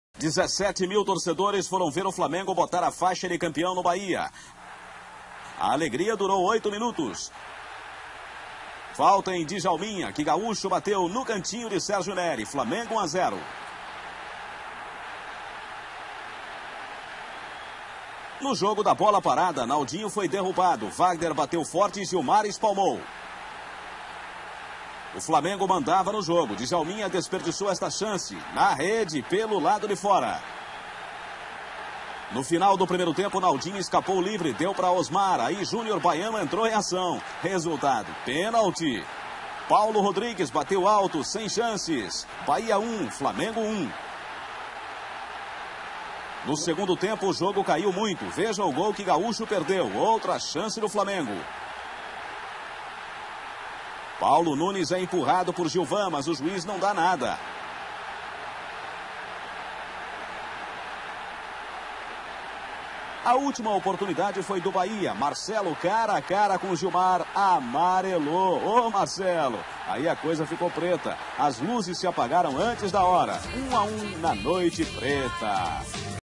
Bahia 1x1 Flamengo - Brasileiro 1992 - Melhores momentos e gols - 29/01/1992
Bahia X Flamengo 92
Campeonato brasileiro 92
Gols
Gaúcho